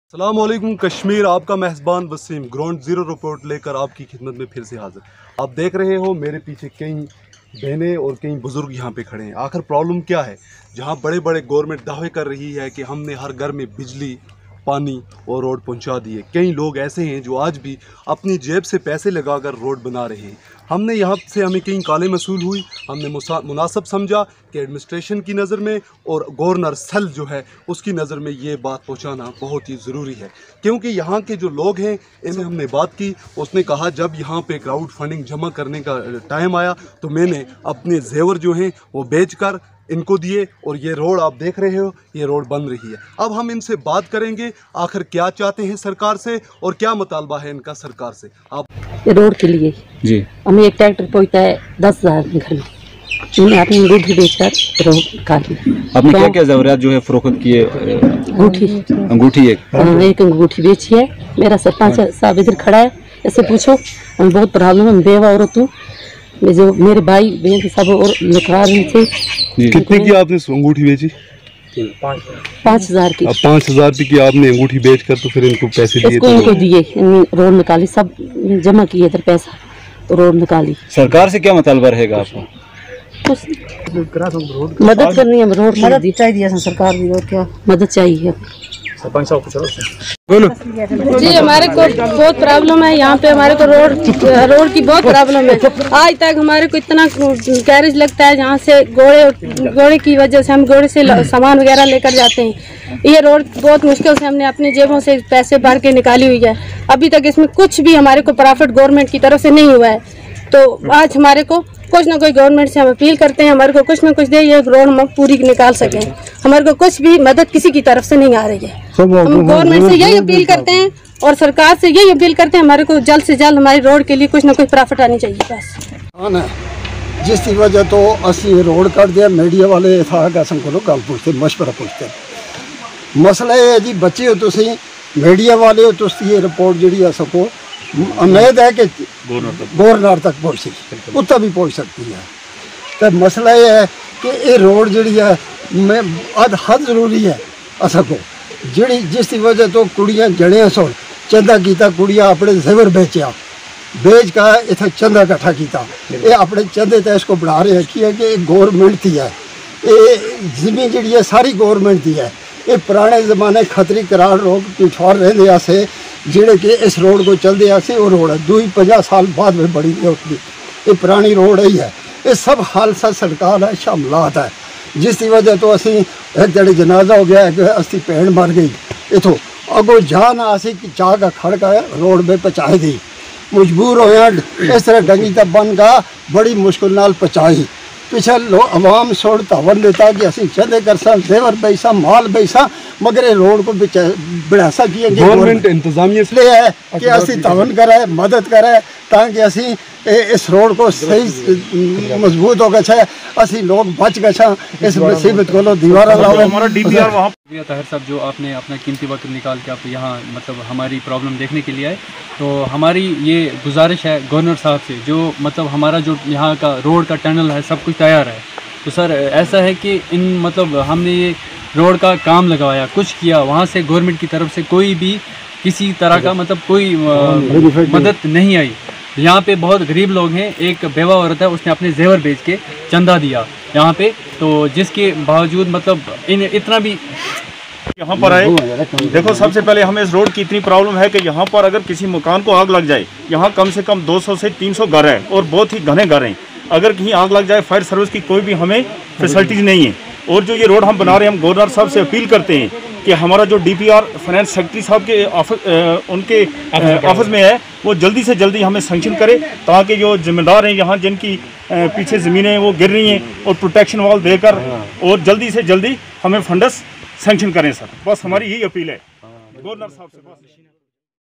The Chitterkote residents generated funds and make the half km Road Widow Woman sell her Gold Ring
The Chitterkote residents generated funds and make  the half km Road Widow Woman sell her Gold Ring to make her Participation in Crowd funding for Road